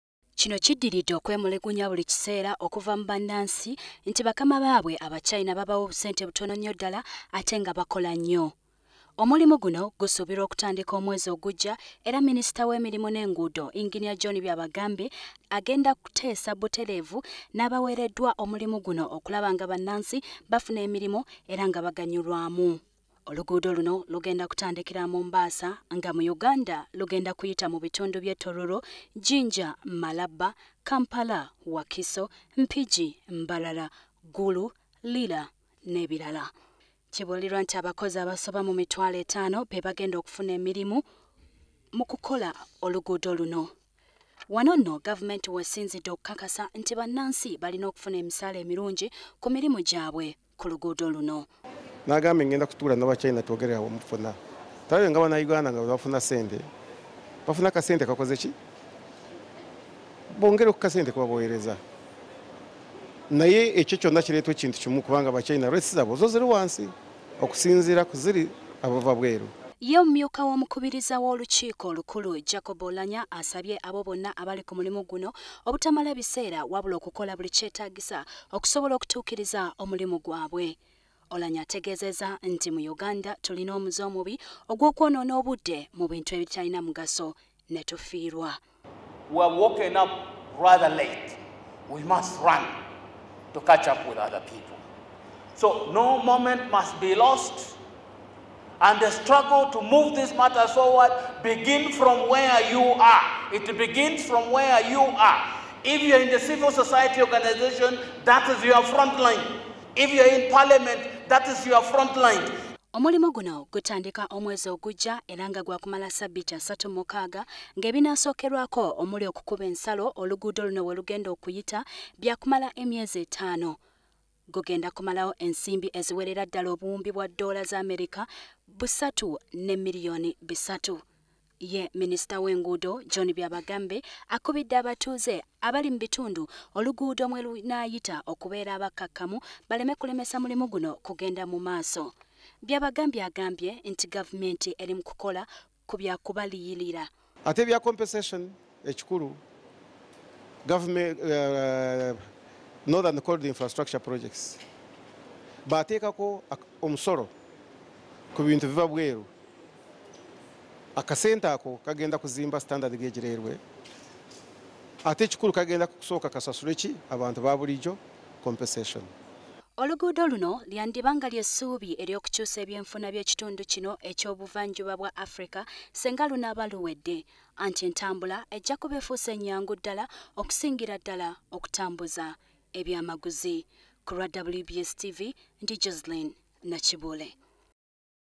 GAVUMENTI YAKUTEESA N’ABA CHINA KU MISAALA GY’ABAKOZI
Gavumenti esazeewo y’eba eteesa butereevu n’ebitongore by’abachina ebigenda okukola oluguugo lw’egali y’omukka ku nsimbi z’abakozi okusingira dala banansi ba Uganda.
Bino minisita w’emirimu n’emirimu Eng John Byabagambi ategezezza bino bwabadde mu kutongoza oluguudo lw’egaali y’omukka wano mu Kampala